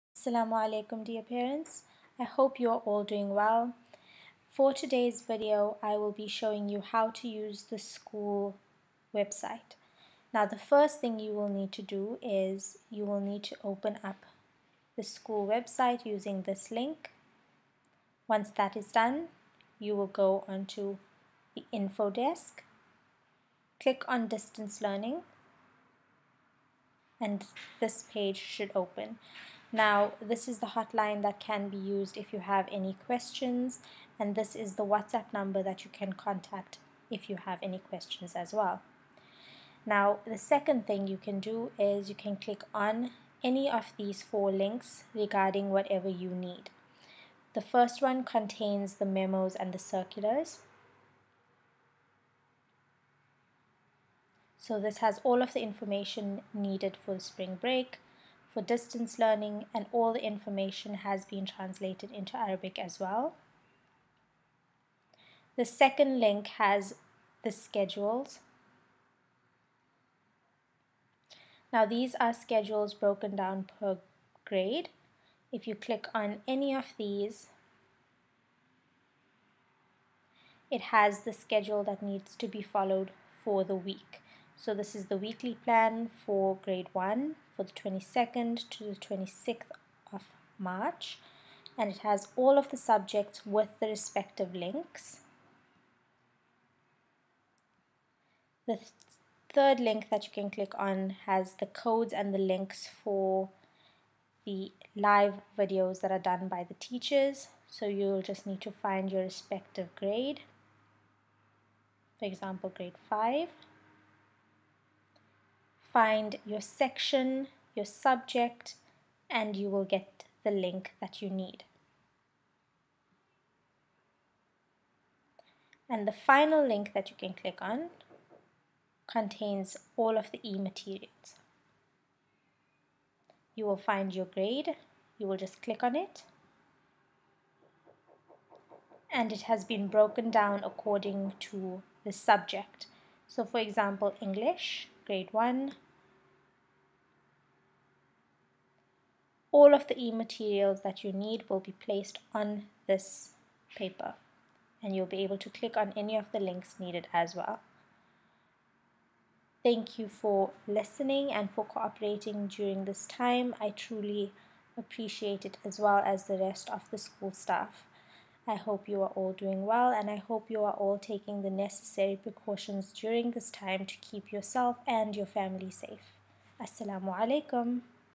AIS - Distance learning (Just a click away)
Dear parents,
This video will guide you to access the distance learning app using only 5 steps. All learning material and related resources will be available for distance learning.